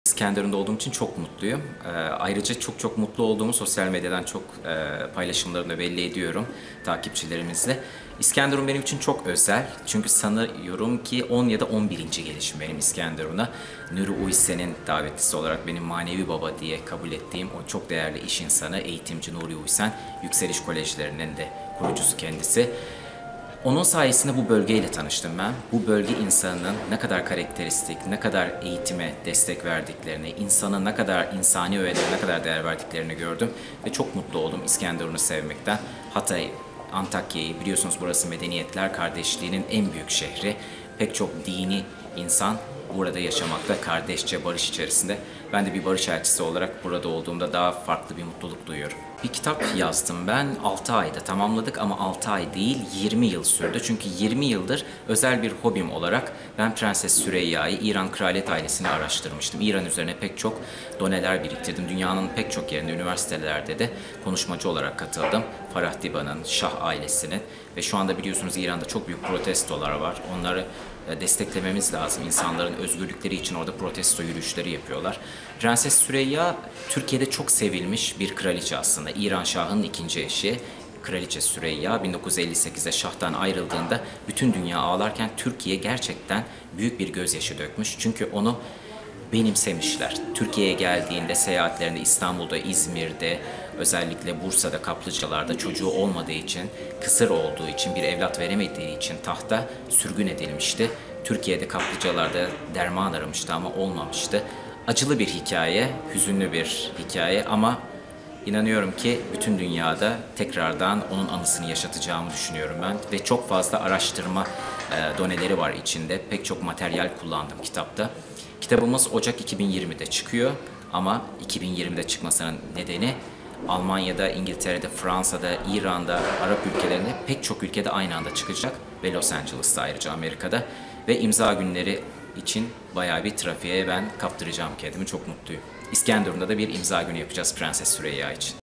İran Kraliçesi Süreyya ile dünyayı sarsmaya hazırlanıyor
Türk Sanat Müziğinin genç seslerinden İzmirli şarkıcı barış elçisi Ersin Faikzade, İran Şahının ikinci karısı Süreyya’nın bilinmeyen yönlerin 20 yıldır araştırdığı ve tarihe ışık tutacak kitabını 6 ayda tamamladığını söyledi.